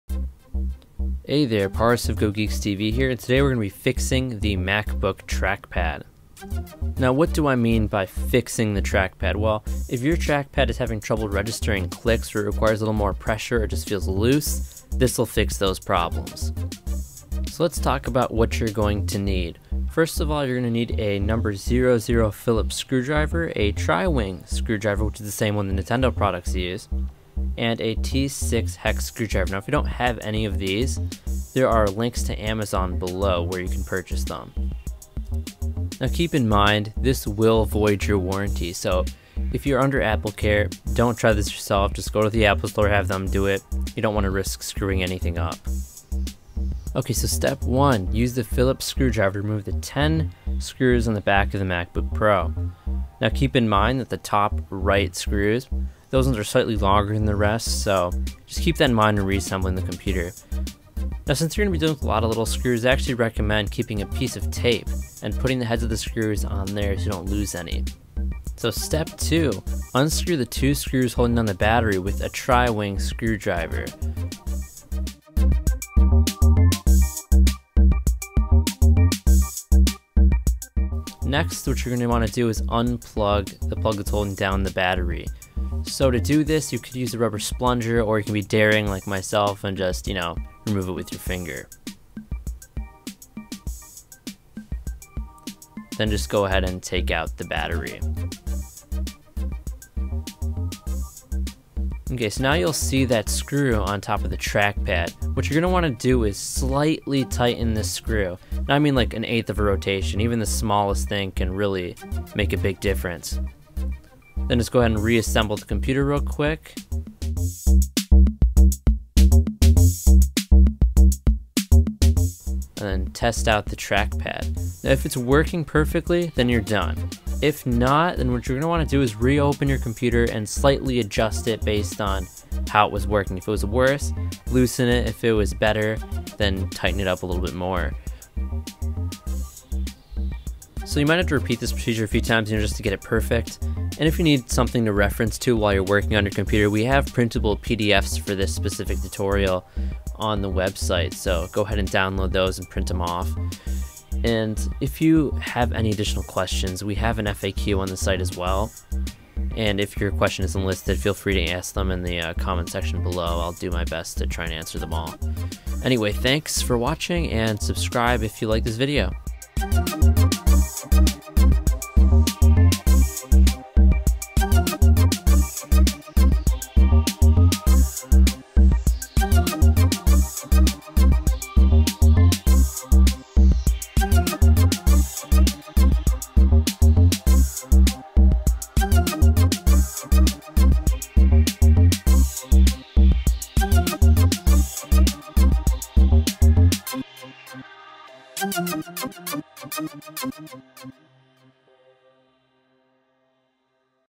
How To Fix Your Macbook Trackpad
This video will show you how to fix your Apple Macbook Trackpad if the click isn't working, requires more pressure than it should (stuck), or if it feels too loose.

Compatible Models:
-Macbook (Late 2008 only)
-Macbook Pro 13" (Late 2008 to Present)
-Macbook Pro 15" (Late 2008 to Present)
-Macbook Pro 17" (Late 2008 to Present)

Tools:
-00 Phillips Screwdriver
-T-6 Screwdriver
-Tri-Wing screwdriver (same as nintendo)

For Debutant441 (this is the torx screwdriver set I use), you should be able to find this same one in your local home depot if you don't want to order online!

Steps:
1. Unscrew the 10 screws on the back and remove the backplate.
2. Remove the tri wing screws holding down the battery
3. Unplug and remove the battery
4. Tighten/Loosen the T-6 Hex Screw
5. Reassemble and test (repeat steps above until perfect)

Note: We are not responsible for any damage you may cause to your computer.

Video by Paras Chadha

how to fix your APPLE MACBOOK PRO MAGIC MULTITOUCH TRACKPAD so it registers all clicks